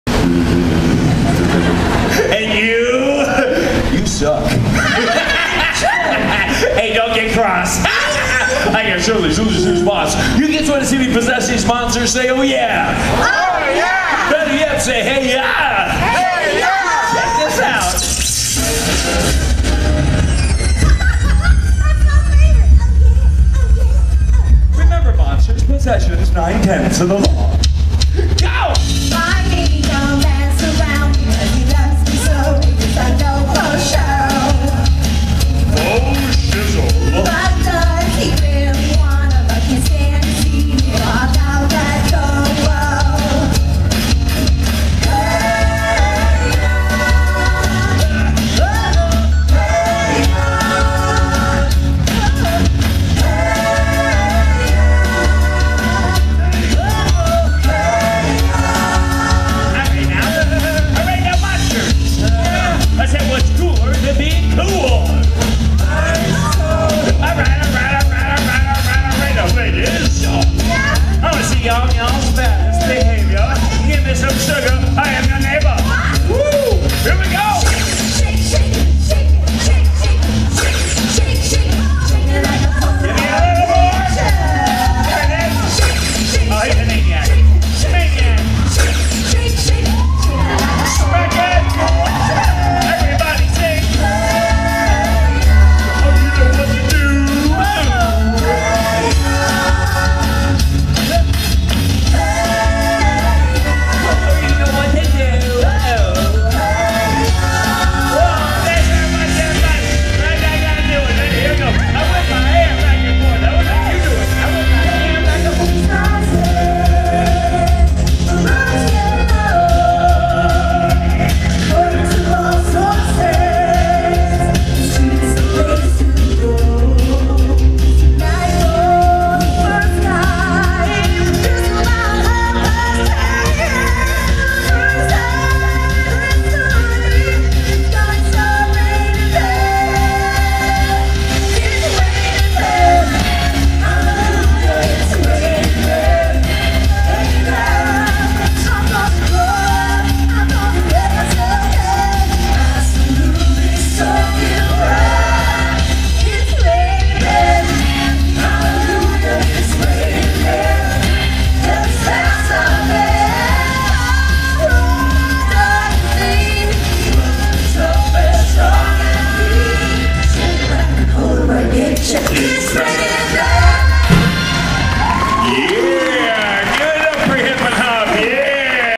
Beetlejuice's Graveyard Revue 10/4/11- FUNNY HOP (Part 4)- HDV 0216
Filmed at Universal Studios, Orlando, FL on October 4, 2011